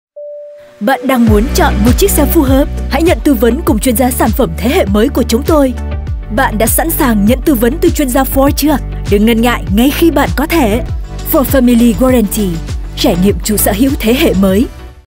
Ford Family Guarantee: Dịch Vụ Tư Vấn Cùng Chuyên Gia Sản Phẩm Ford | Ford Vietnam
Ford Family Guarantee trân trọng giới thiệu đến các chủ sở hữu xe Ford Thế Hệ Mới "chuyên gia tư vấn sản phẩm" - đội ngũ được đào tạo bài bản và chuyên sâu về kiến thức sản phẩm, cũng như kỹ năng chăm sóc khách hàng để luôn sẵn sàng hỗ trợ bạn.

Đặt lịch ngay tại hoặc gọi đến tổng đài Ford Việt Nam qua hotline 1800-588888 (miễn phí cước cuộc gọi) để trải nghiệm.

Tìm hiểu thêm về cách chúng tôi lắng nghe, cam kết và hỗ trợ bạn tại